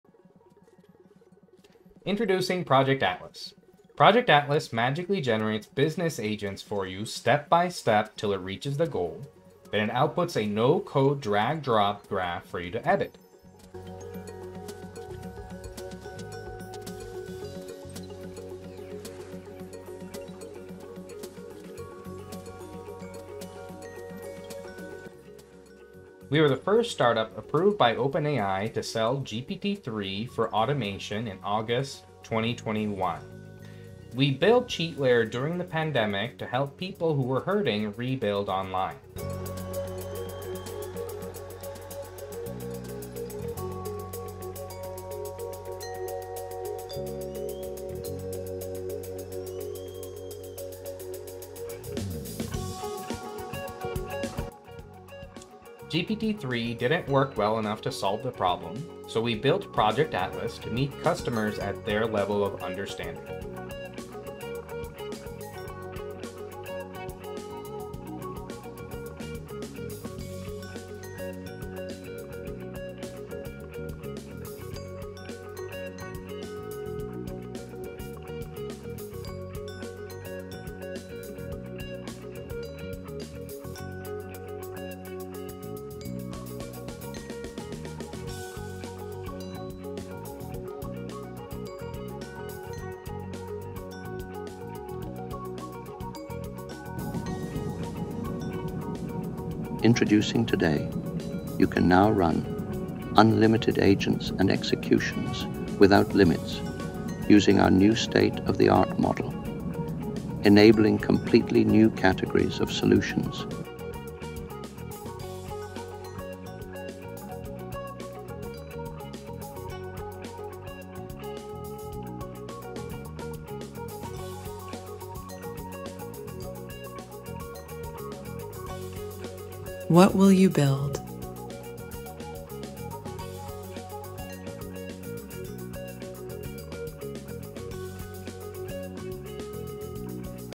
Project Atlas - Magically generate desktop no-code agents from end-to-end in natural language.
Project Atlas generates complex business automation agents step-by-step using a state machine which considers the current state of the screen and assets to take actions towards the goal until it's complete. It then verifies the output using a tight testing loop with another verification agent.